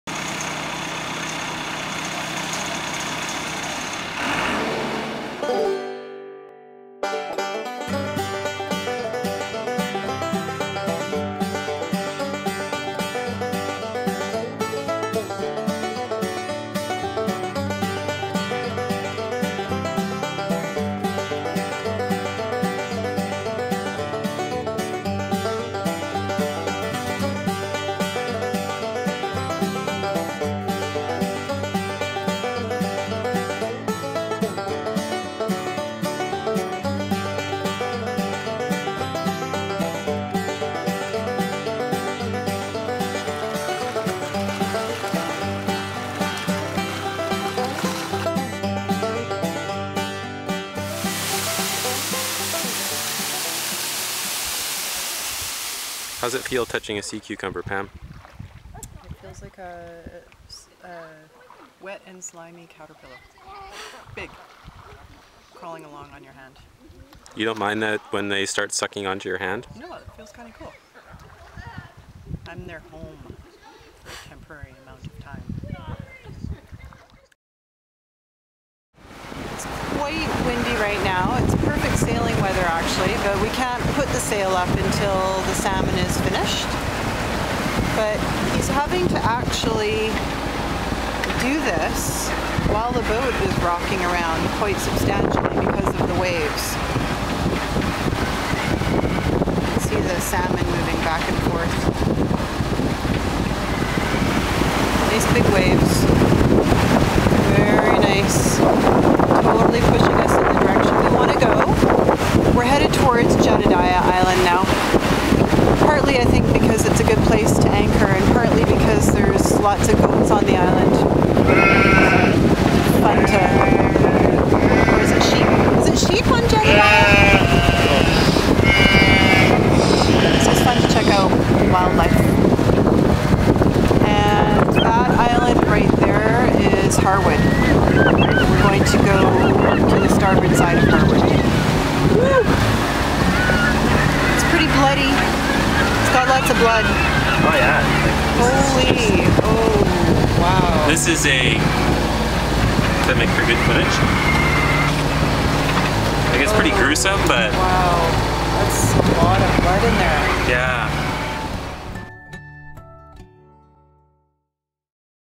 Best Boating Times 2010-2
Transcript:
Granville Island Boat Yard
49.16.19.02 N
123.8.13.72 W
a clean bottom is a fast bottom
Thanks for taking me boating Dad!
first day with our new dinghy
going places you can only go on a boat
How does it feel touching a sea cucumber Pam
It feels like a wet and slimy caterpillar, big, crawling along on my hand
You don't mind when they start sucking onto your hand?
No, it feels kind of cool, I'm there home for a temporary amount of time
It's quite windy right now, it's perfect sailing weather actually, but we can't put the sail up until the salmon is finished
when possible we practise the zero mile diet
But he's actually having to do this while the boat is rocking around quite substantially because of the waves
You can see the salmon moving back & forth
Very nice big waves, totally pushing us in the direction we want to go
We are heading towards Jedidiah Island now, partly because it's a good place to anchor and partly because there's lots of goats & sheep on the island.
free range sheep are descendants of those left by Spanish explorers
It's fun to check out the wildlife
And that island right there is Harwood, we are going to go to the starboard side of Harwood
It's pretty bloody, it's got lots of blood
This makes for good footage, it's pretty gruesome
Oh, wow there's lots of blood in there
Yeah
offering sailboat charters on the salish sea